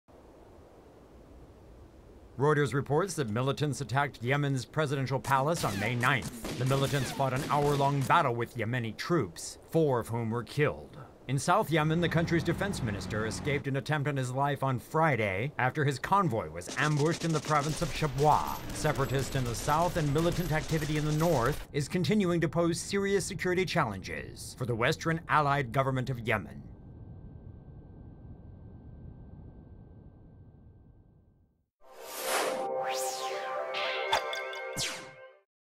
Militants attack Yemen presidential palace
Reuters reports that militants attacked Yemen's presidential palace on Friday, May 9.

The militants fought an hour-long battle with Yemeni troops, four of whom were killed.

In south Yemen, the country's Defense Minister escaped an attempt on his life on Friday, after his convoy was ambushed in the province of Shabwa.

Separatists in the south and militant activity in north Yemen is continuing to pose serious security challenges for the Western-allied government of Yemen.

Reuters reports that since late April, the Yemeni government has stepped up its campaign against the terror group considered al Qaeda's most active unit known as al Qaeda in the Arabian Peninsula (AQAP).

Military campaigns have driven the AQAP out of what had been strongholds in the south and the attack on the presidential palace is seen as pushback from the militants.

The terrorist attacks come following air strikes in April that the government said killed at least 55 militants, in the biggest military action against al Qaeda for several years.